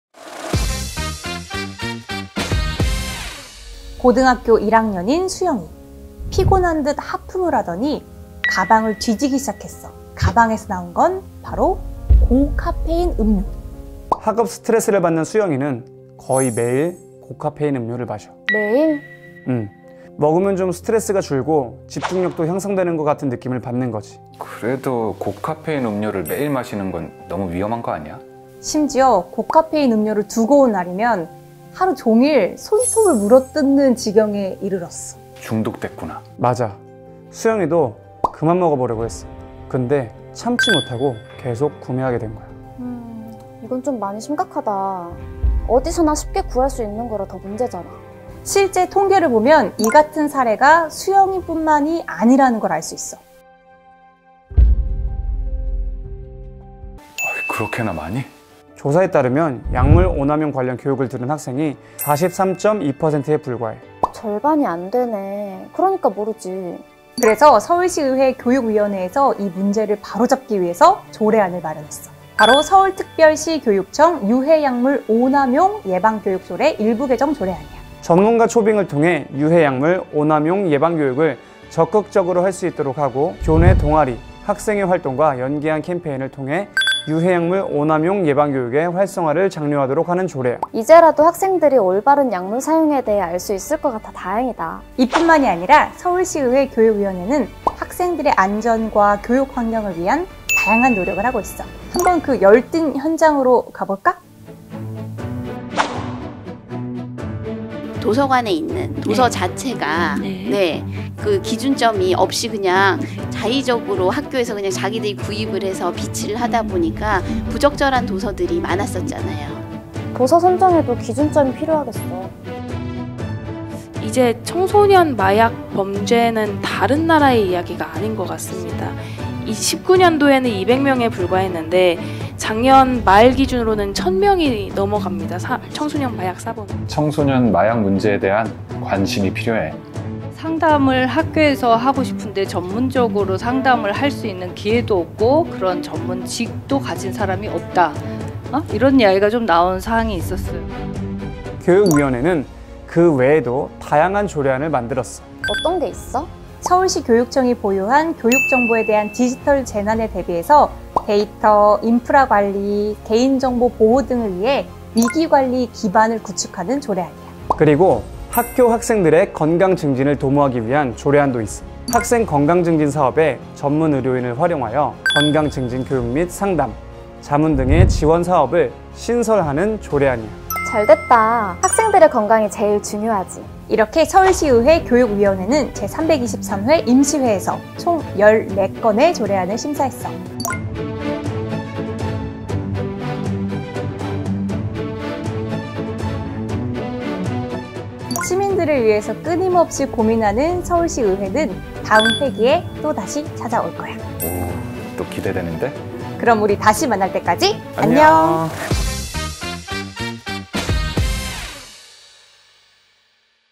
치명적인 💊유해약물 오·남용, 이제 ⛔그만!ㅣ🗣상임위썰ssul 제323회 임시회 교육위원회
서울특별시의회 제323회 임시회 기간 중 교육위원회는

🗨'서울특별시교육청 유해약물 오·남용 예방교육 조례 일부개정조례안'을 통해 유해약물 오·남용·예방교육을 적극적으로 할 수 있도록 하고

🗨'서울특별시교육청 디지털재난 대비 및 대응 조례안'과 서울특별시교육청 학생 건강증진에 관한 조례 일부개정조례안 을 포함한

총 14건의 안건을 심사하여 서울시 학생들의 안전과 건강에 이바지하고 있습니다.